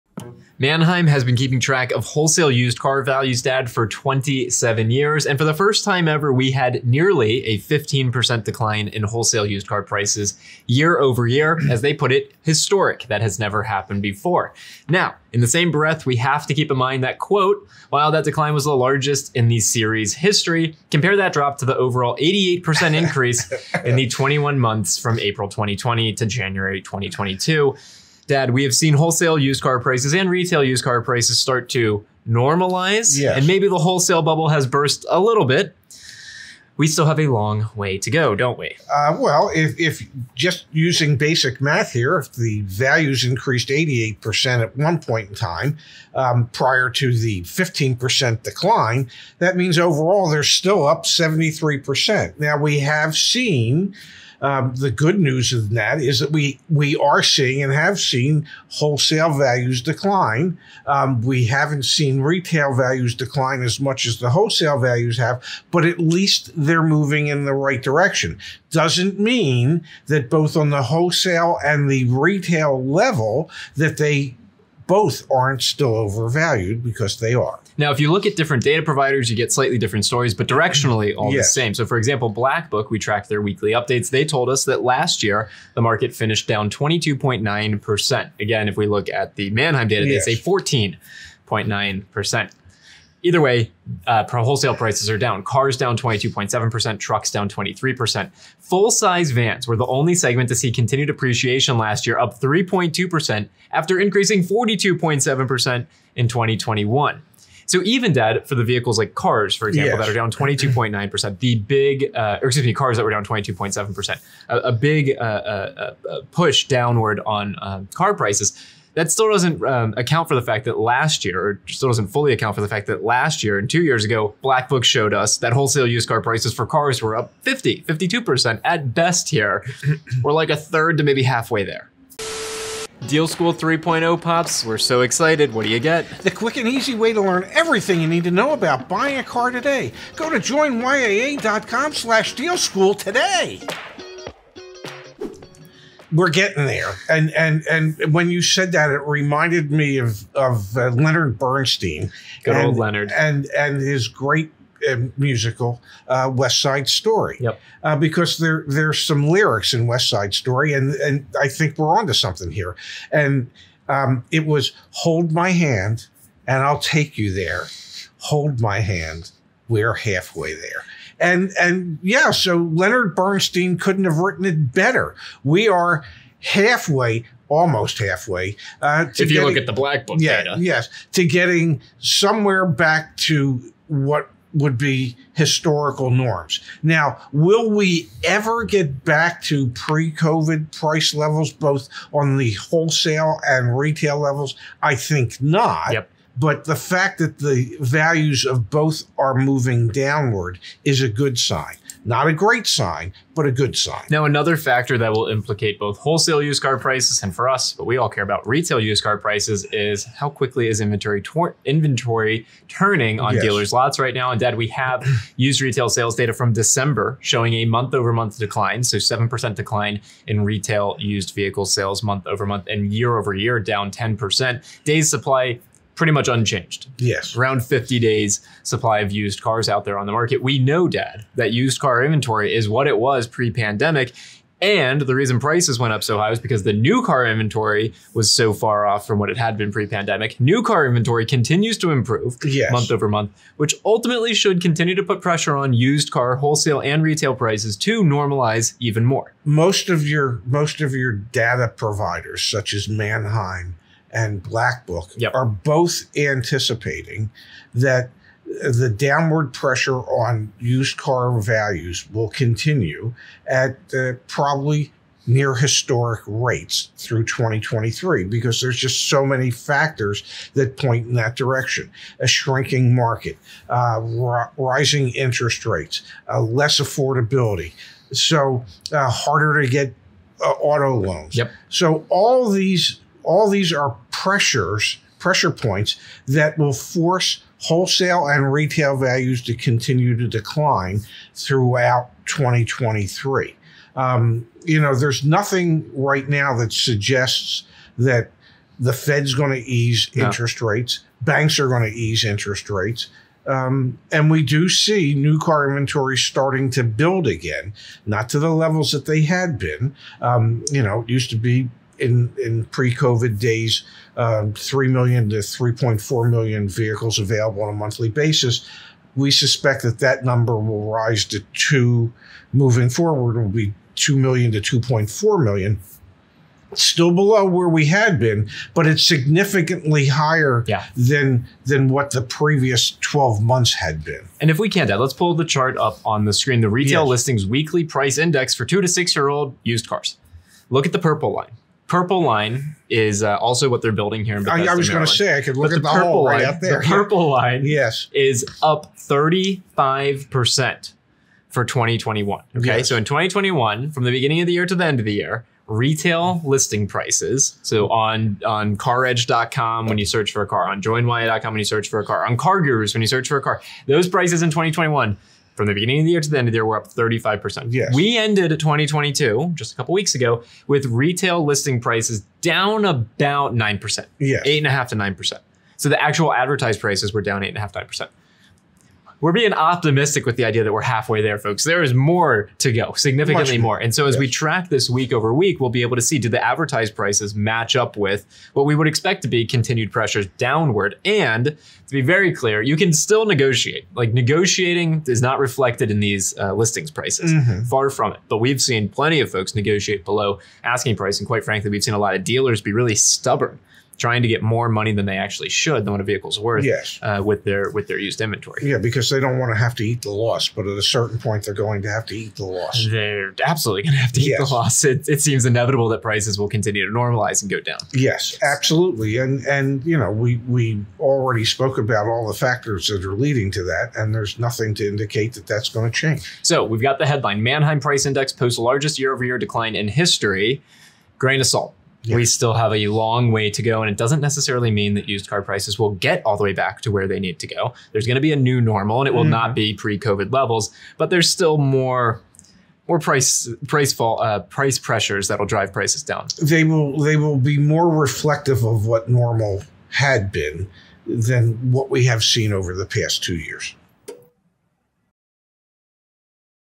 Used Car Prices ARE STILL OVERPRICED | CRASH INCOMING
Ray and Zach take a retrospective view of the used/new car markets in 2022. Although used wholesale/retail prices fell significantly (with new car inventories on the rise), we've still got a ways to go until the market returns to anything resembling the way it was pre-pandemic.